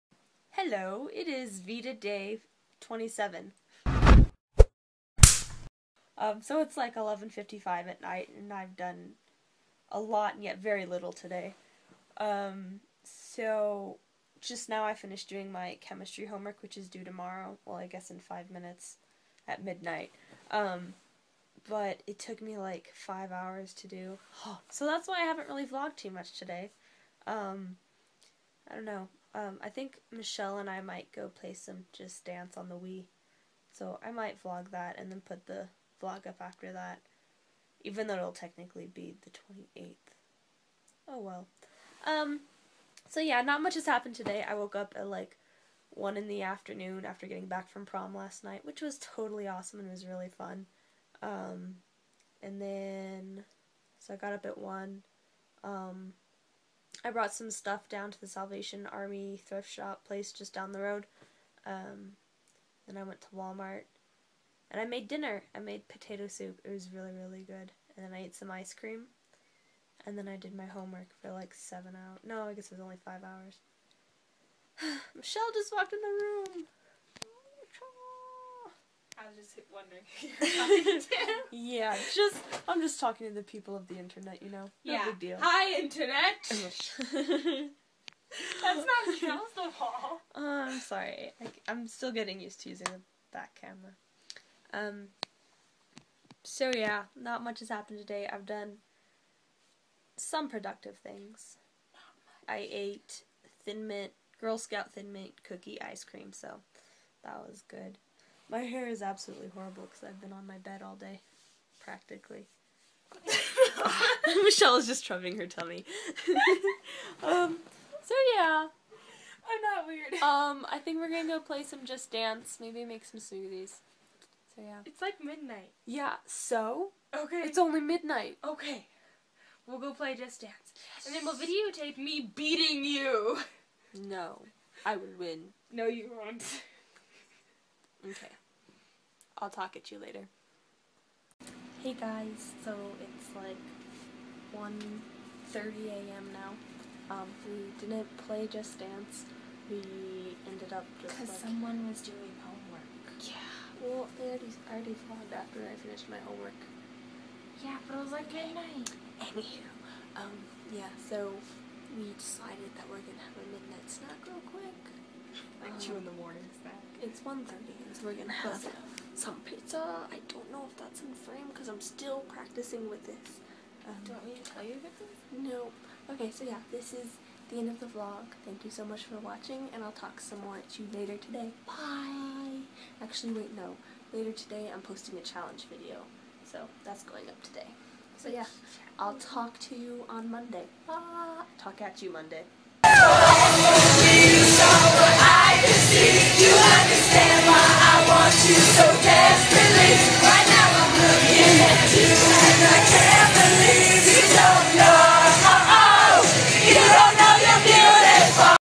VEDA Day 27 - Midnight Snack
So today was kind of a lazy day. I spent most of my time doing
homework. :/